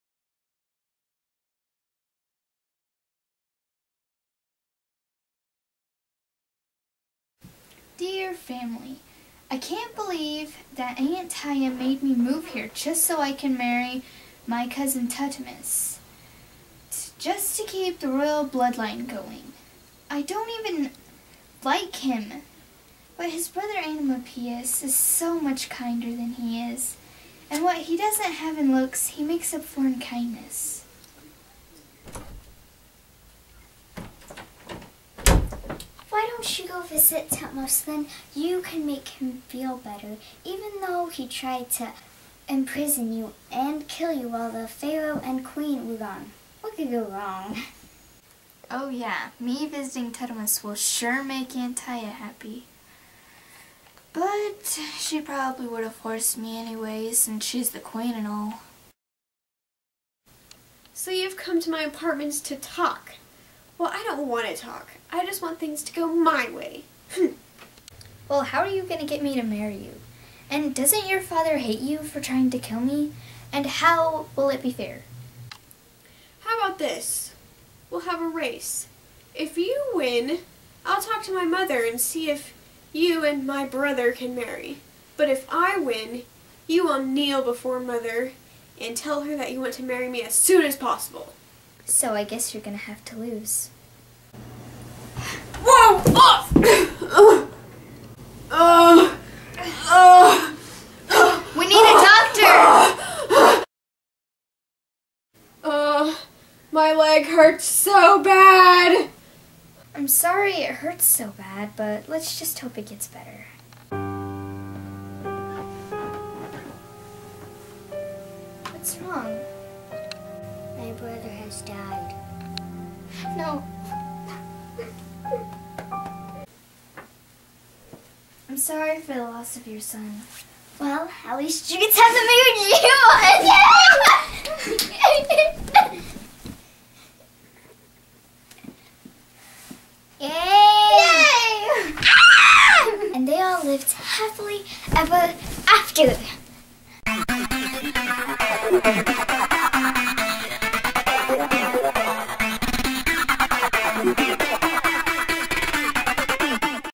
Sphinx's Queen (Book Report)
Bernadette needed a visual aid to go along with her book report for school, so here it is! It is a summary of the book "Sphinx's Queen" by Esther Friesner. We hope you enjoy this video, even if you haven't read the book!